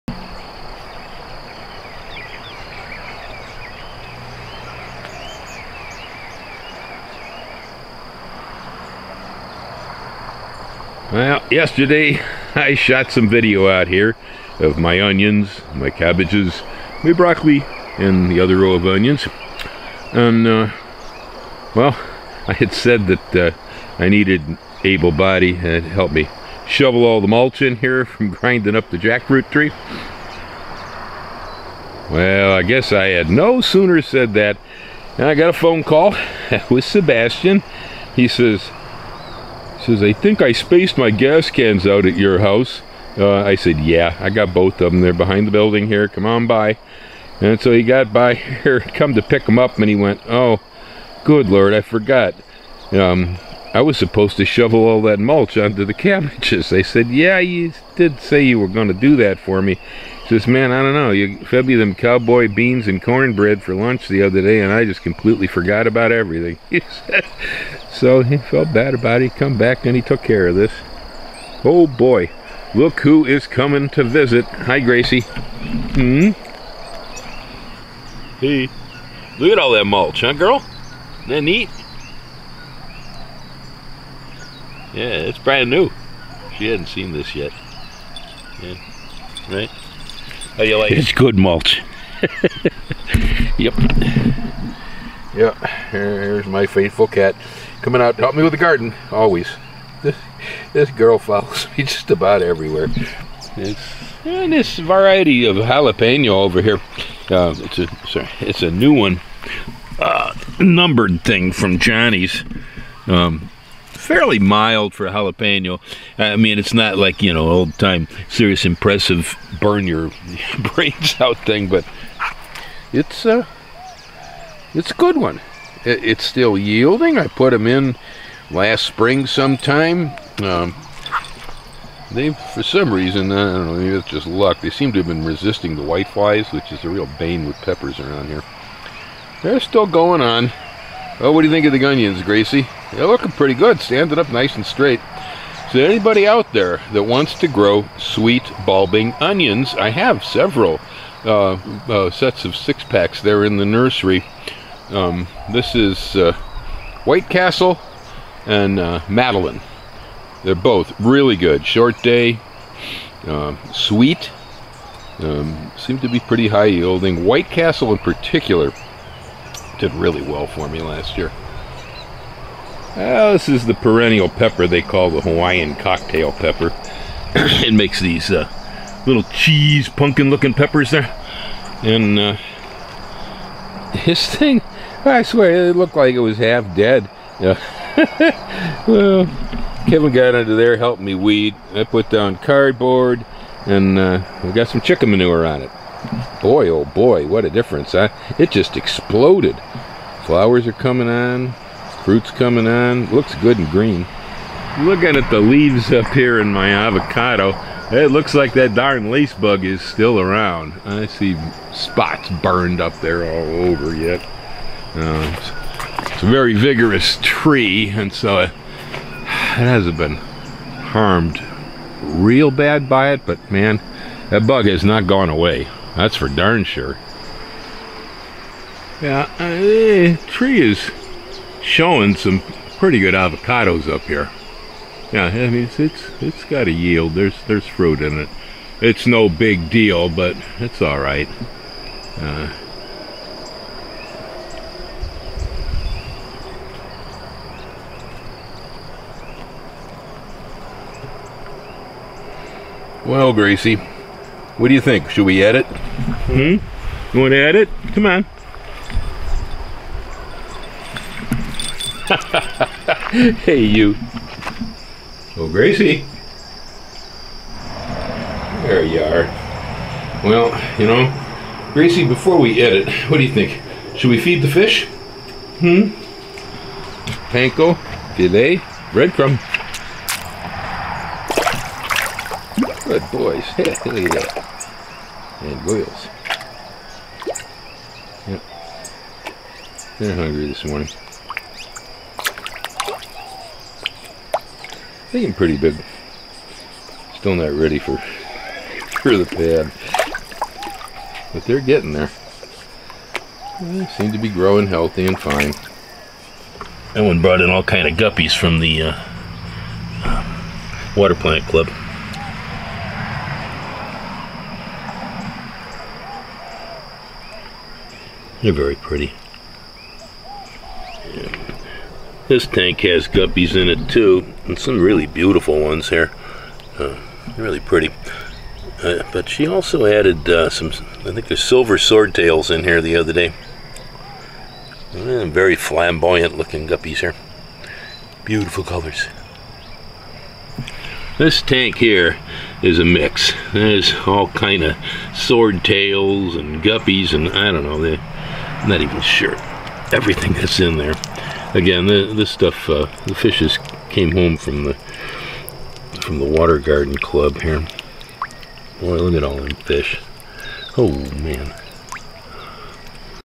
Mulching the Vegetables
Bill finishes mulching the new vegetable planting and checks on Avocado Lace Bugs.